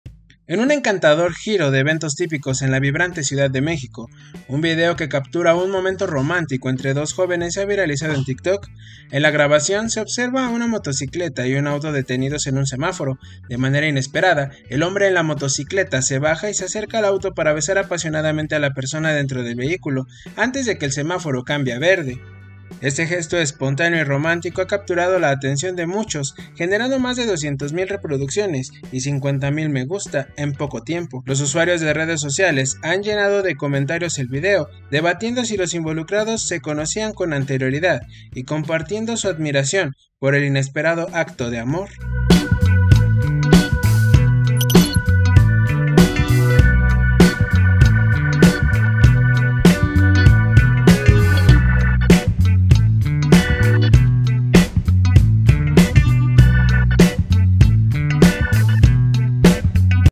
Joven aprovecha el alto para coquetear con conductora y ¡SE AGARRAN A BESOS!
La Ciudad de México esconde detalles sorprendentes en cada una de sus calles por ser una de las metrópolis más vibrantes y diversas del mundo. Por ello, no debe sorprendernos que un par de jóvenes decidieran aprovechar el alto para cruzar miradas, coquetear un poco y darse un beso antes de que el semáforo cambiara a verde.